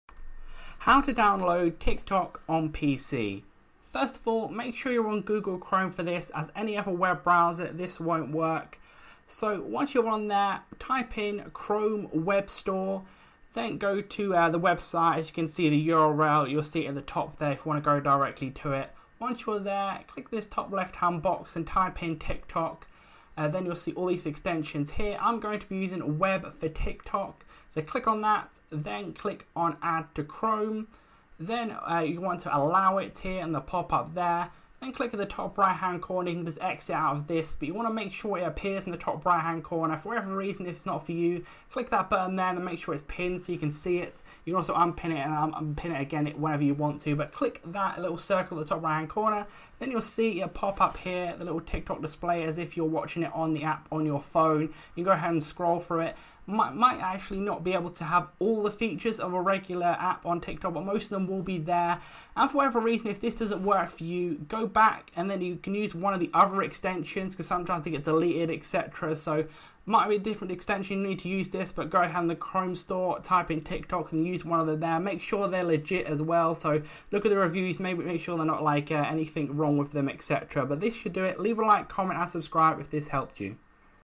How to Download TikTok on PC (How to Install TikTok on Laptop) *2021 NEW UPDATE*
How to Download TikTok on PC (How to Install TikTok on Laptop) *2021 NEW UPDATE*. Step by step instructions of how to get tiktok on your computer for windows 10.